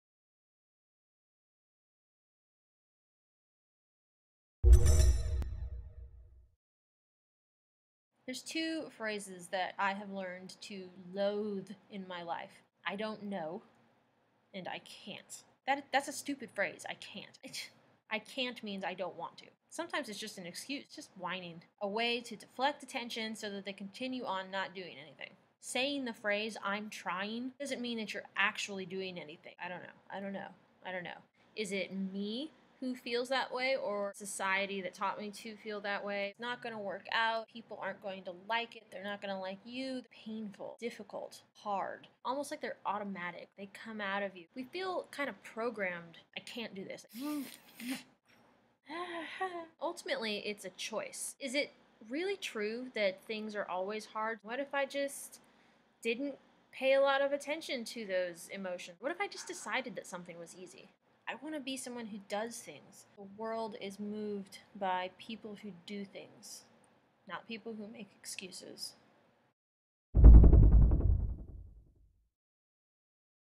Full Circle: What's holding you back? - Shayla Maddox
Exploring the thinking that holds us back. "I'm trying." "I can't." "It's too difficult." What if it's easier than we think?